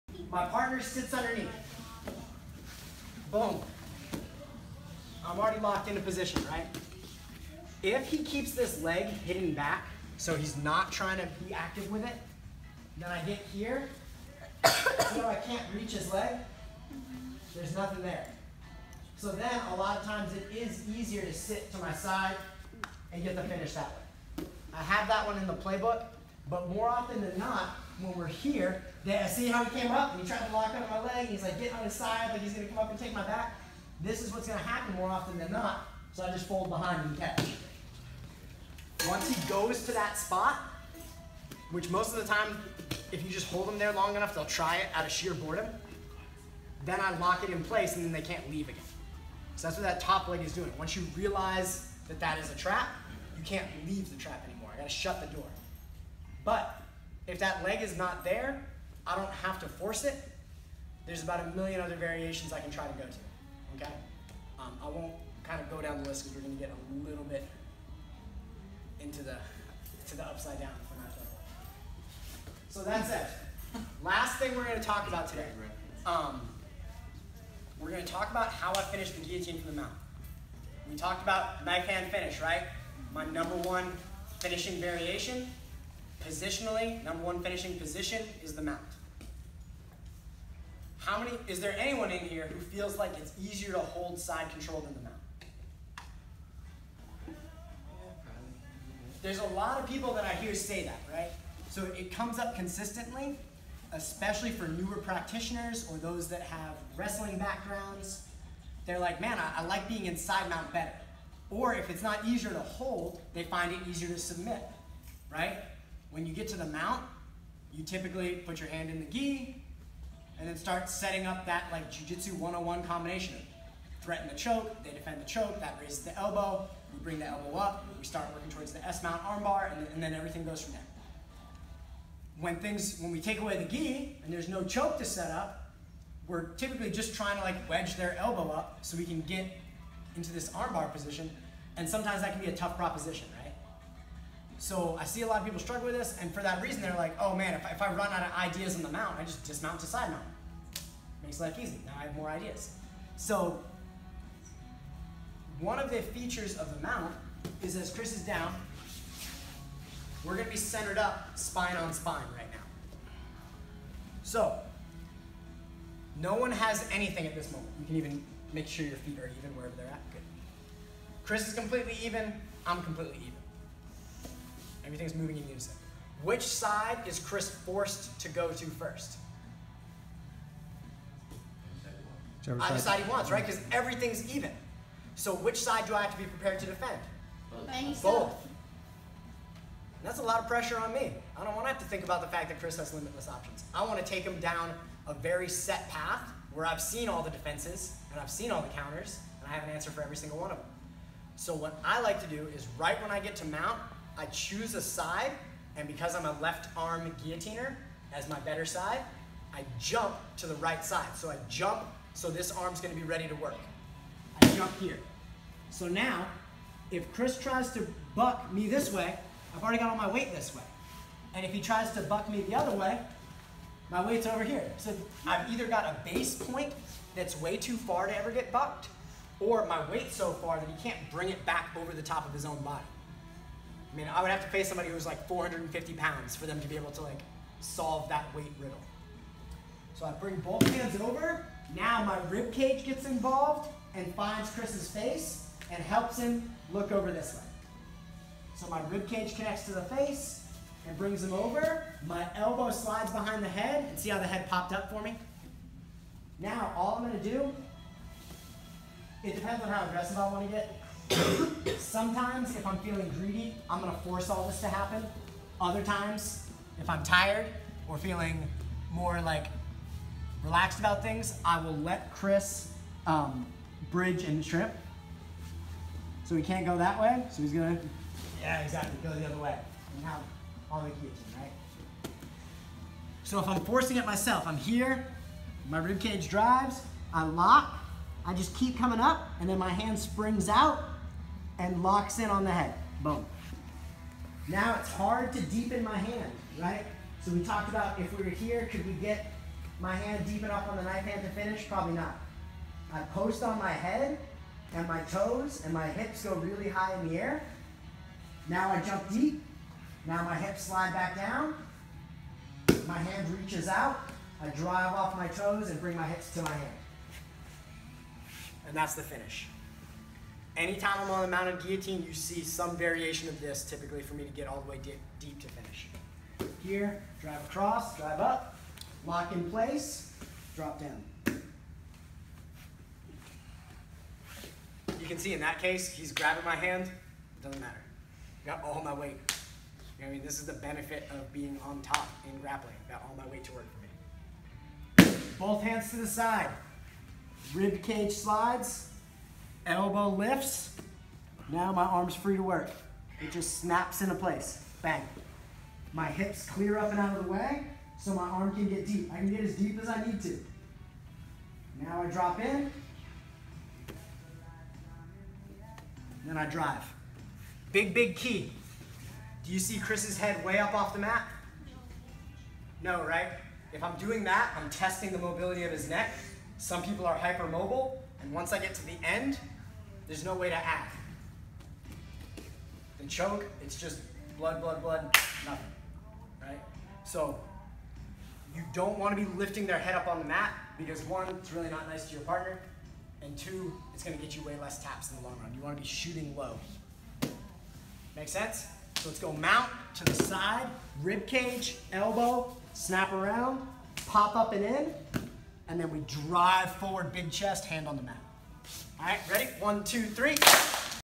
Setting Up the Knife Hand Mount Guillotine
This is my #1 finish and how I generally play once mounted.